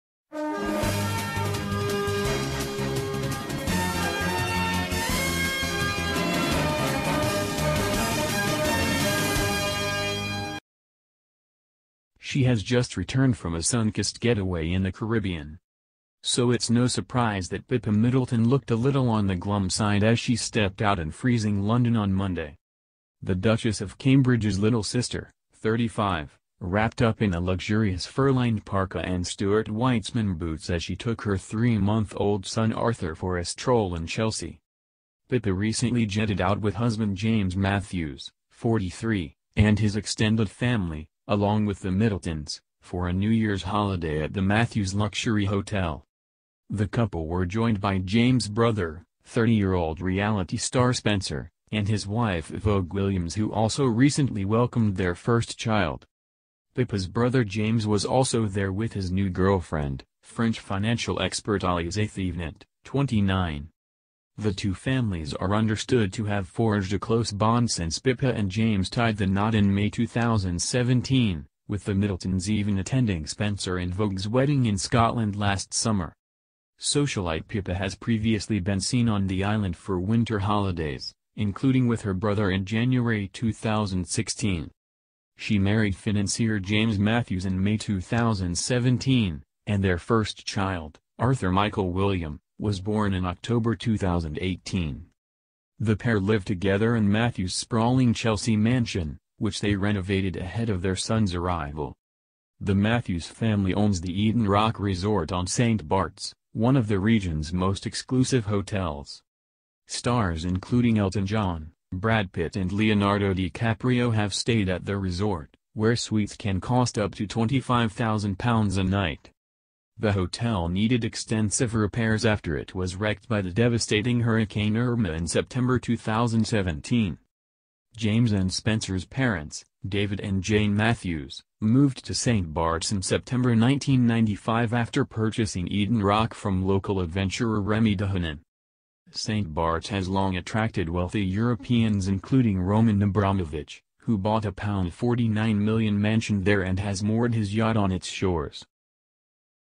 Pippa's spotted back in London with baby Arthur for the first time since her Caribbean getaway
Pippa's Blue Monday! New mother wraps up in a luxury fur-lined parka as she's spotted back in London with baby Arthur for the first time since her Caribbean getaway with the in-laws